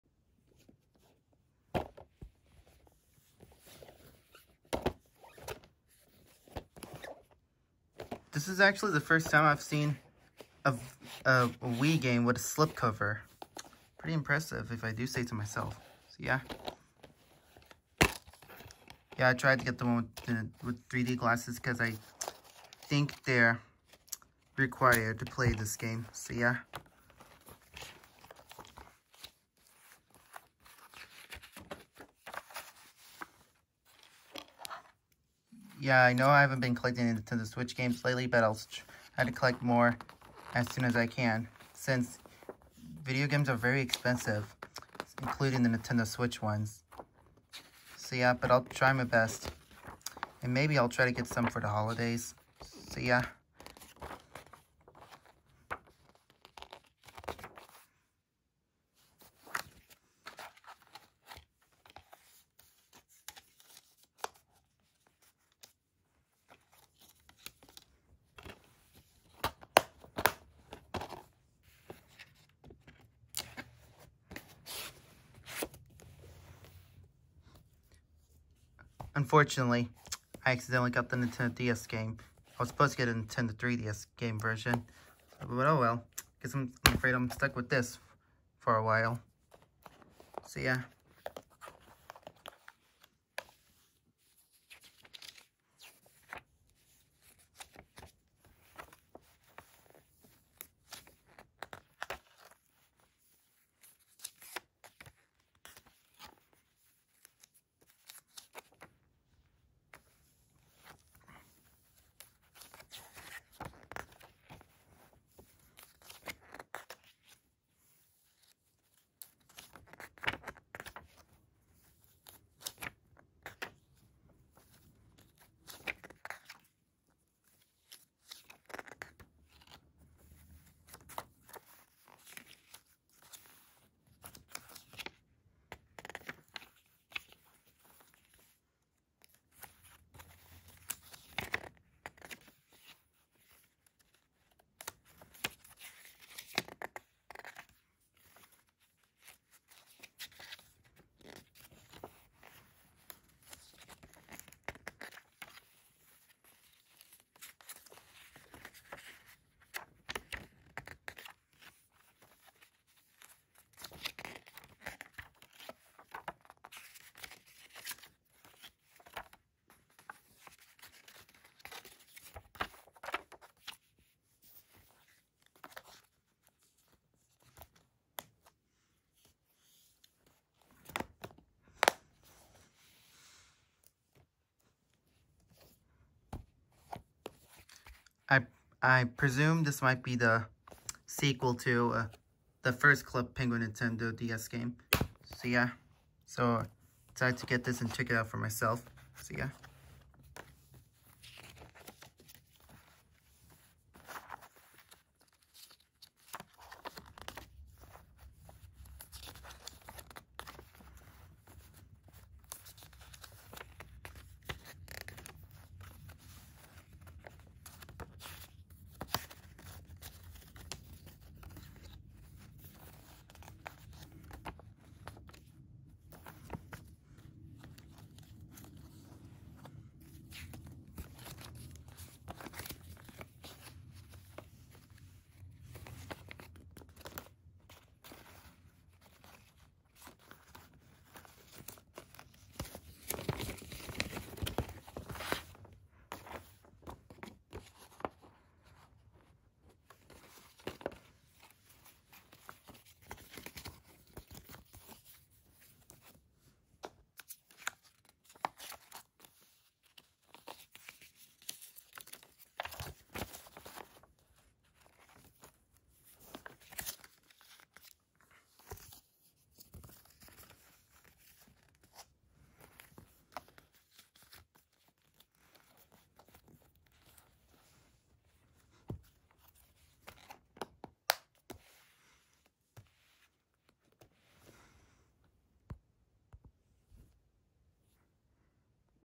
Video game update(read description)
I might have a hard time finding the Nintendo 3DS version of Mario Luigi Bowsers inside story but I’ll see what I can do ￼